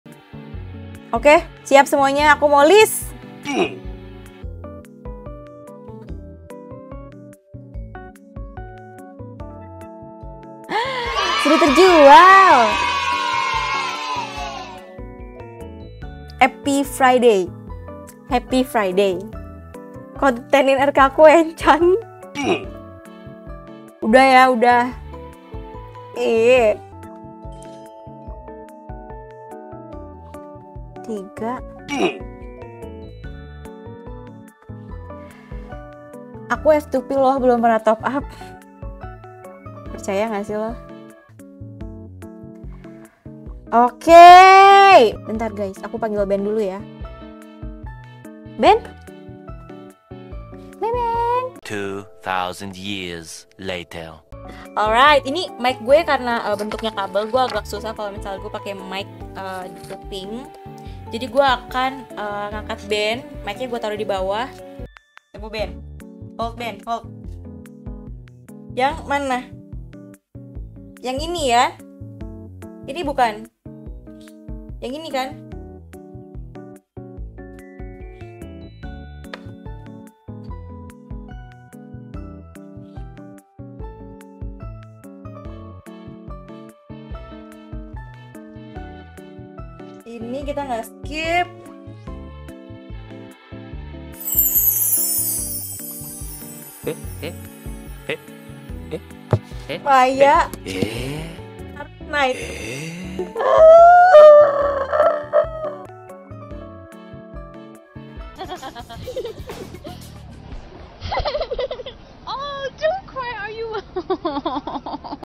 GACHA MVP CARD UNTUK VIEWER TERCINTA
Kali ini Azab kembali menantang kaki Ben untuk memencet layar. Bagaimanakah hasilnya?

Jangan lupa nanti malam kita seru2an lagi di live jam 21.00!